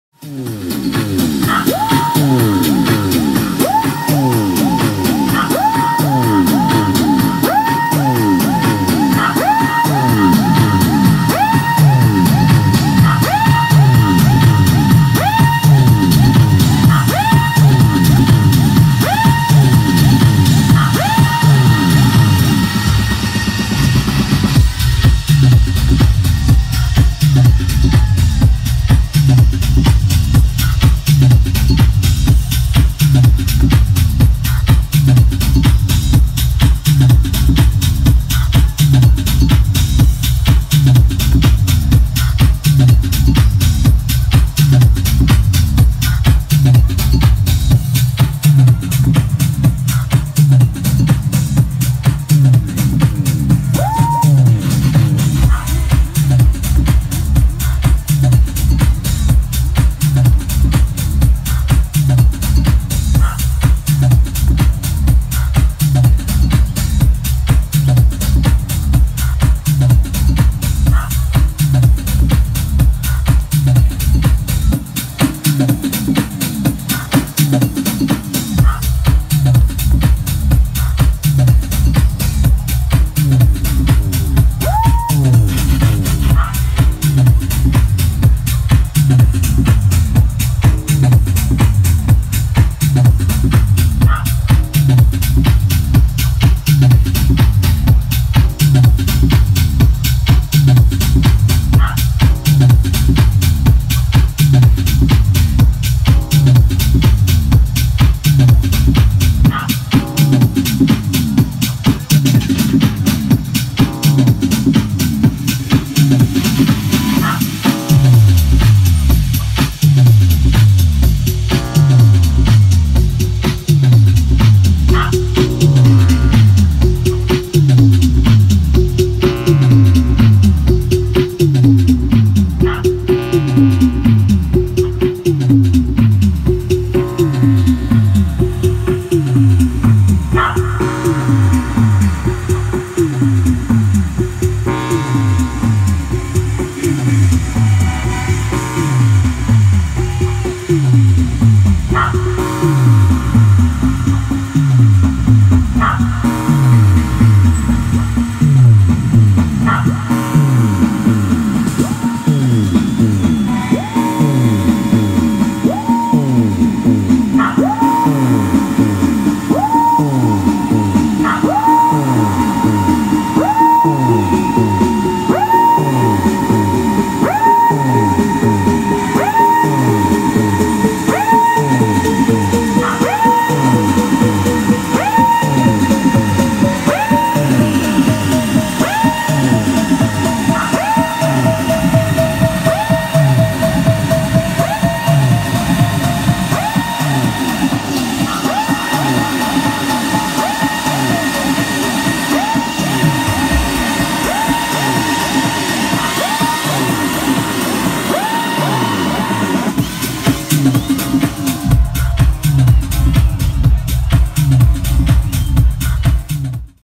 Fer BR @ Hidrosun Festival Pres. Mindshake 9th July 2017(Puerto Alcudia)
Mindshake showcase :
Paco Osuna
Fer BR
German Brigante
Manu Sanchez